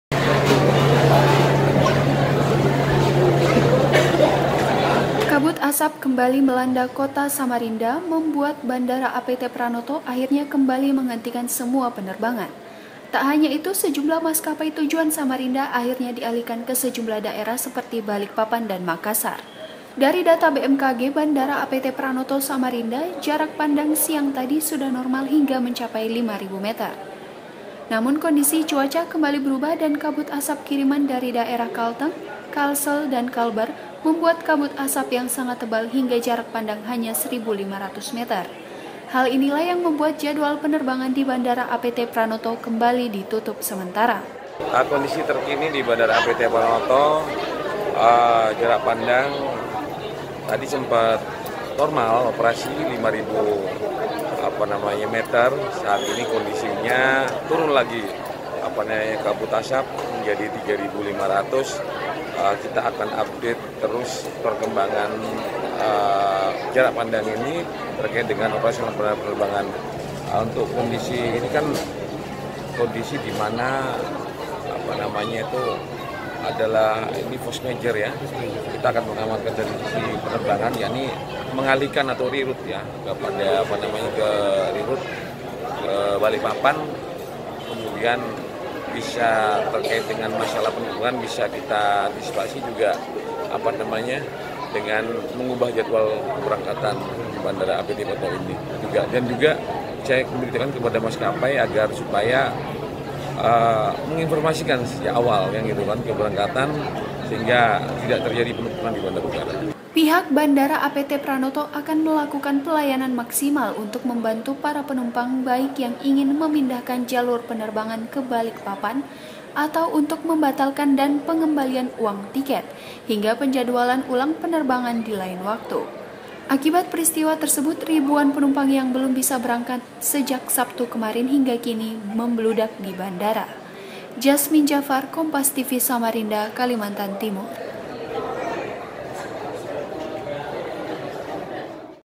Bandara APT Pranoto Samarinda Kembali Ditutup Sementara
Kabut Asap yang Melanda Kota Samarinda Membuat Bandara APT Pranoto Kembali Menghentikan Seluruh Penerbangan. Tak Hanya itu, Seluruh Penerbangan Menuju Samarinda Akhirnya Dialihkan Ke Beberapa Daerah Seperti Balikpapan dan Makassar. Ribuan Penumpang Akhirnya Menumpuk Di Bandara.